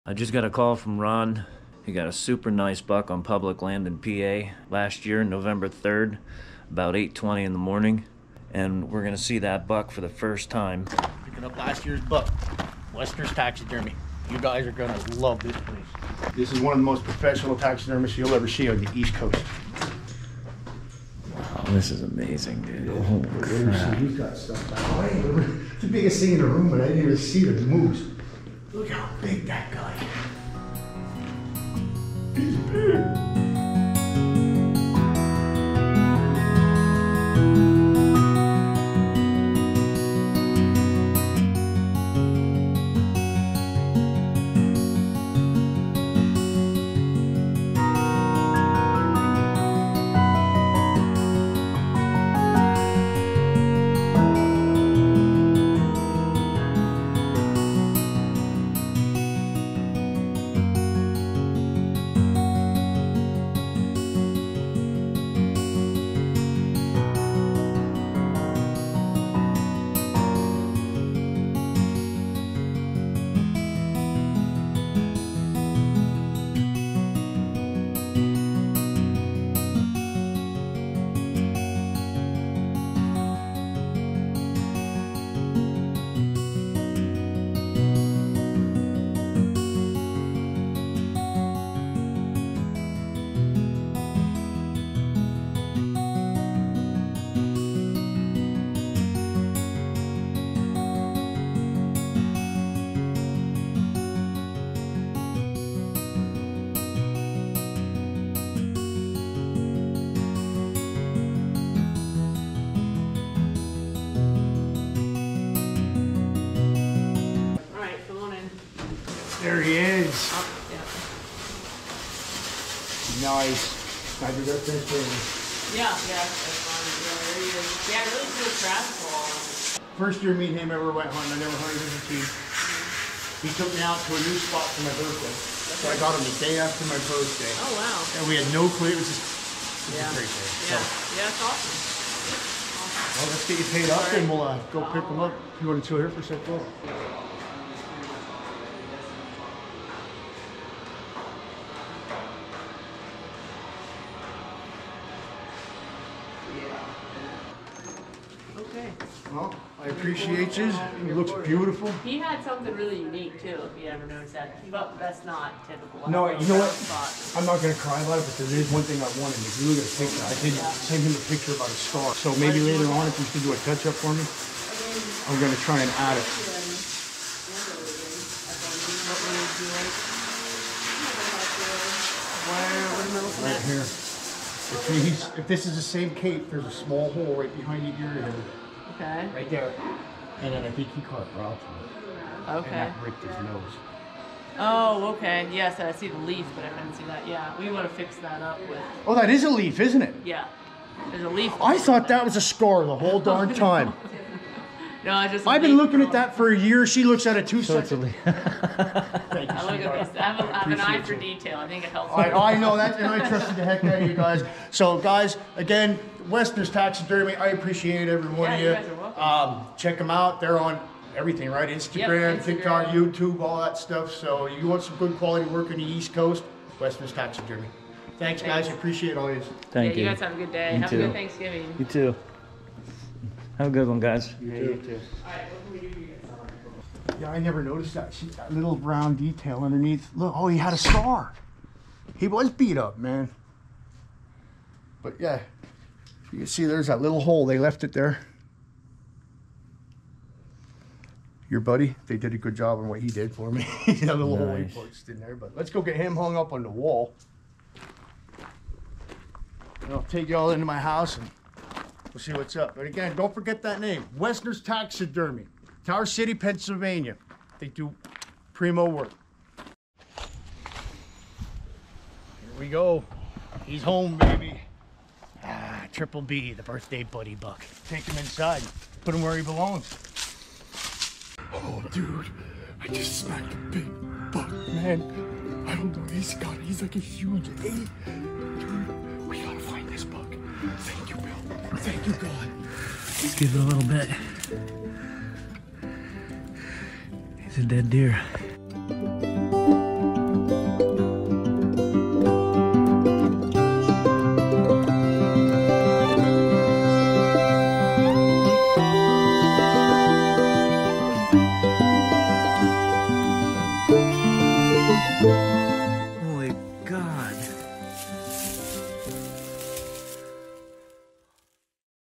Picking Up The Rut Buck Shoulder Mount from the Taxidermy
We picked up the shoulder mount buck, from Rahns Harvest during rut. One of the Best wild life creations and taxidermist on the east coast. The long awaited anticipation has come to an end. What a cool story behind this buck! One of the biggest memeries of our lives. Wessner's Taxidermy does awsome work! Staff and owner are very friendly. What a great group of people to be around. Follow this story as there are a few on the channel. Rahn and Bill hunted together for the first time and it was a successful year for archery season. Bill needed a partner and dedicated this year to his late father who passed the year before. Miss you pops, I know you're watching over me with the new hunting partner, I'll guide everyone from here on out. Keep watching. @wessnersoutdoors.com,